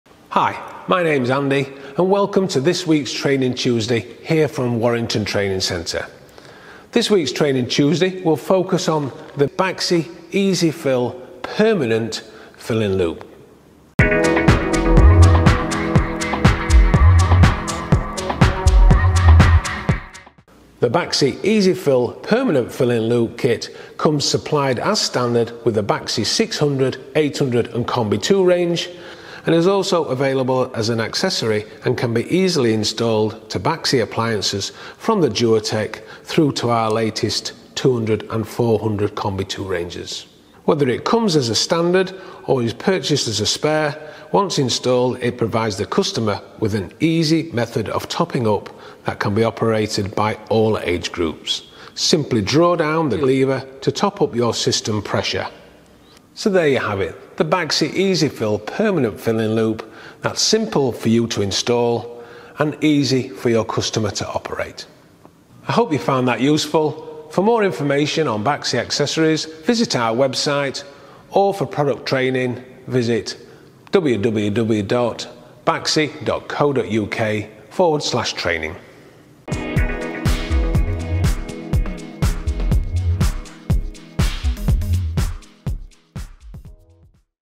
A closer look at the Baxi Multifit Easy-fill 2 Permanent Filling Link
In this edition of Training Tuesday, Baxi trainer Andy takes a closer look at the Baxi Multifit Easy-fill 2 Permanent Filling Link/Loop which is available as a Baxi Genuine Part and allows the end user to top up their boiler central heating pressure via a spring-loaded green lever, quickly and easily – negating the need to call out a heating engineer, saving time and hassle.

Please note: This video is intended for competent heating and hot water engineers only, for example must be Gas Safe registered if working with gas, or have the Unvented Hot Water Qualification (G3) and is subject to our full video disclaimer which can be found here: